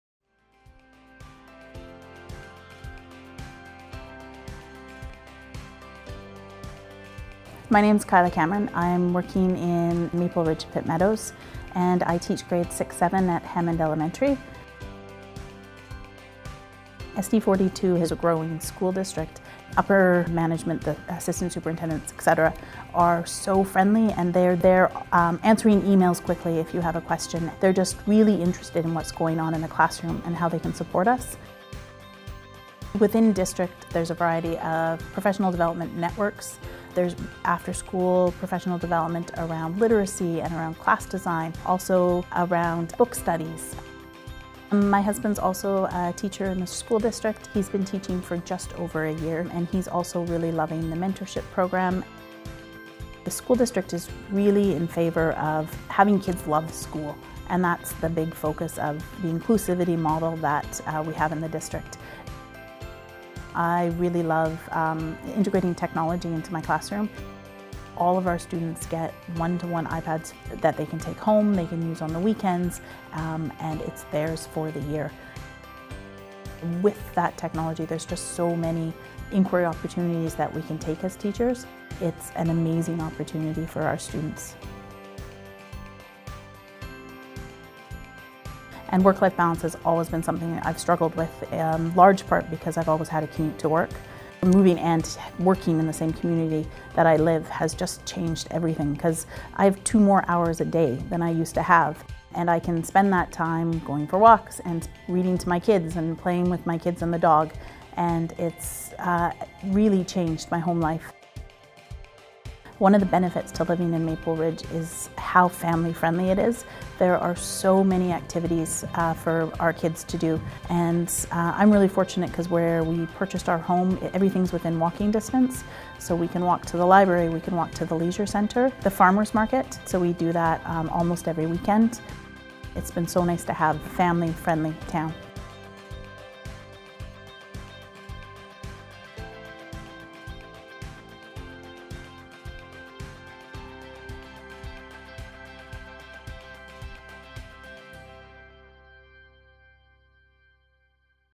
Meet the BC Teacher - Kyla Cameron - School District 42 - Maple Ridge-Pitt Meadows
Meet elementary teacher, Kyla Cameron and learn about her experience living in Maple Ridge and as an employee at School District 42.

Find a full list of School District 42's job listings